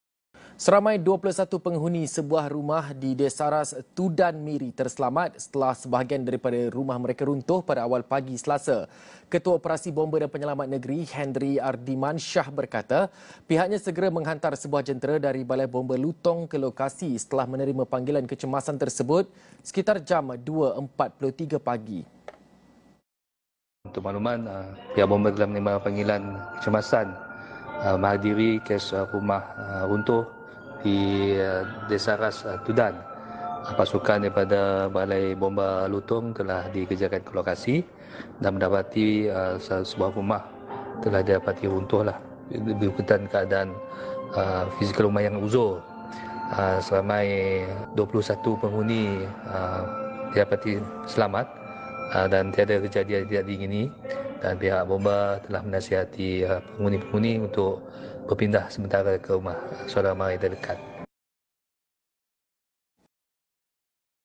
RISIKO ROBOH | 21 PENGHUNI DI DESARAS TUDAN DIARAH PINDAH
Seramai 21 penghuni sebuah rumah di Desaras Tudan, Miri terselamat, setelah sebahagian daripada rumah mereka runtuh pada awal pagi Selasa.

Sumber : @🇲🇾Astro AWANI🇲🇾